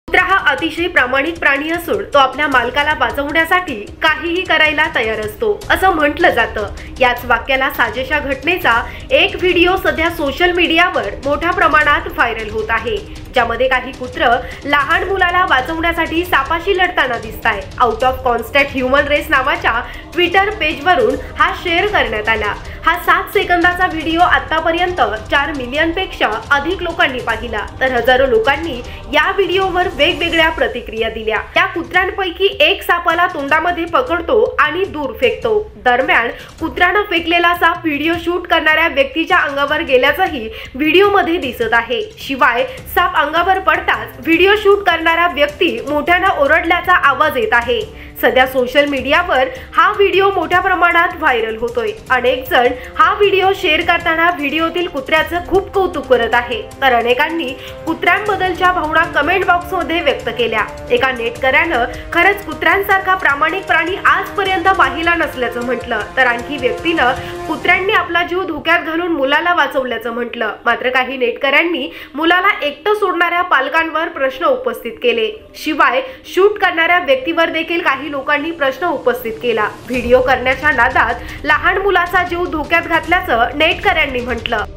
लहान मुलाकडे येत होता विषारी साप; कुत्र्यांनी क्षणात धाव घेतली | snn marathi । viral video
लहान मुलाकडे येत होता विषारी साप; कुत्र्यांनी क्षणात धाव घेतली | snn marathi । viral video snn_marathi marathi_news viral_video viral_on social_media